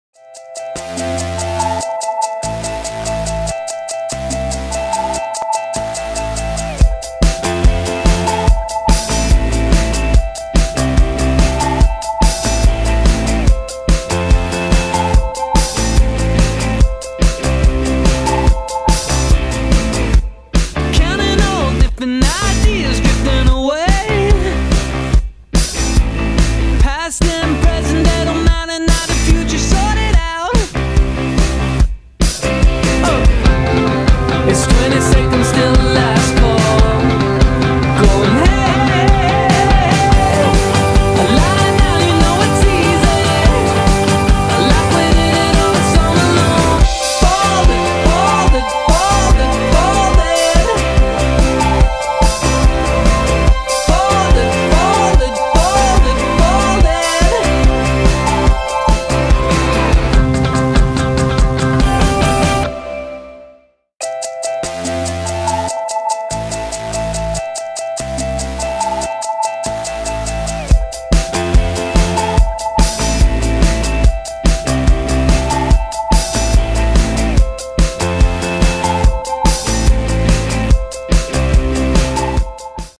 Saturday of fun
Green & Gold playing toypedo game

(Created and edited on iPhone 4 with iMovie)